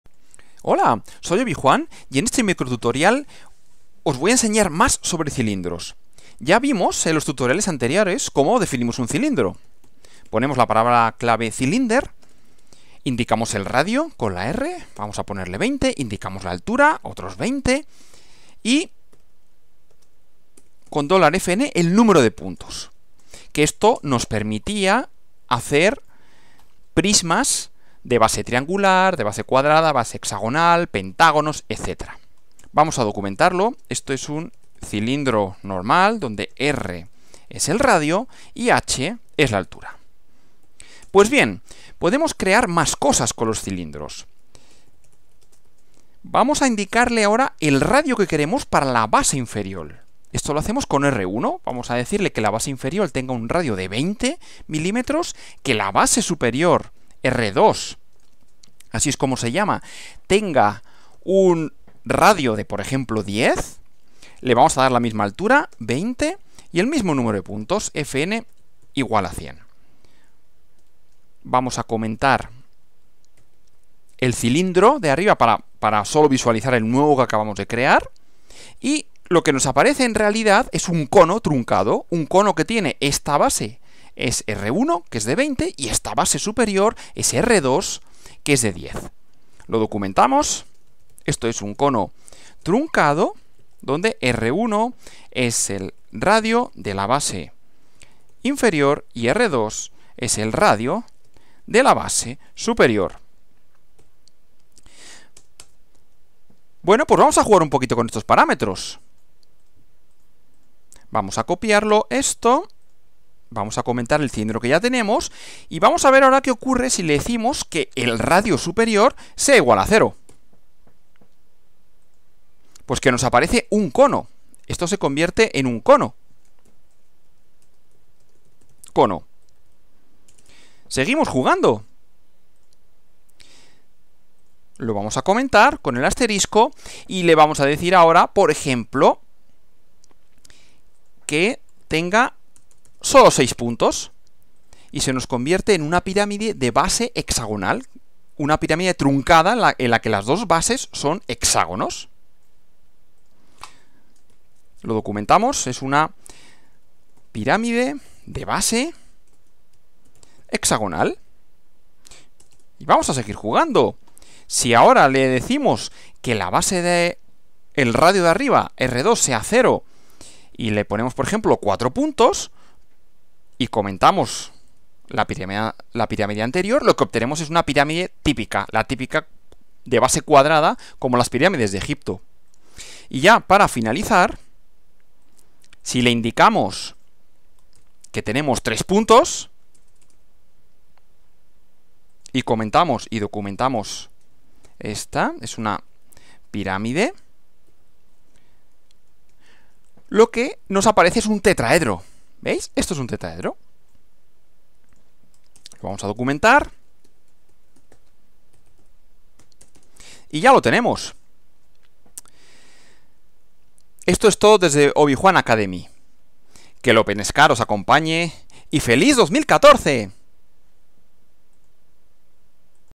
Tutorial Openscad 2: Las pirámides son cilindros! (2/28)
Temporada 2 del tutorial de diseño de piezas con opescad

Cómo crear pirámides y conos usando cilindros